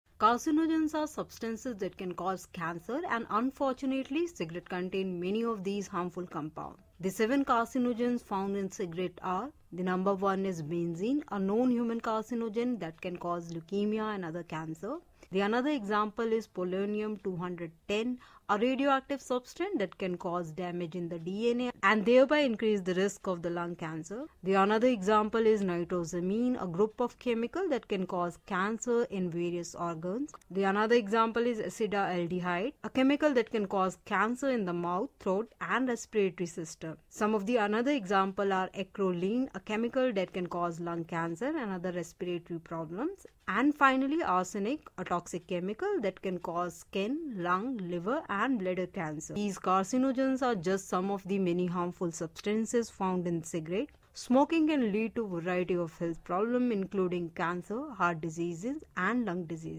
Harmful Carcinogens in Cigarettes - What We Need to Know| Cigarette Smoking and Cancer
Harmful Carcinogens in Cigarettes - What You Need to Know| Cigarette Smoking and Cancer

Hey All,
Are you intrested in biotech, cancer biology and cancer research and wonder how smoking impacts your health.
Are you aware of the seven dangerous carcinogens found in cigarettes? Smoking is a leading cause of cancer, heart disease, and lung disease. In this informative video, we'll discuss the seven harmful substances present in cigarettes, including benzene, polonium-210, nitrosamines, formaldehyde, acetaldehyde, acrolein, and arsenic. We'll also explore how smoking can lead to a variety of health problems and why quitting smoking is the best way to reduce your risk of developing these diseases.

Don't miss out on this must-watch video!